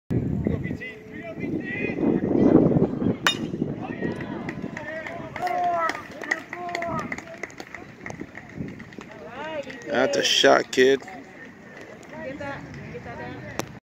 Crossroads C/O 2021 Pitcher Hits a Shot!
Crossroads C/O 2021 pitcher Brendan Terry sends one deep in a preseason game vs Buckley, March 31, 2021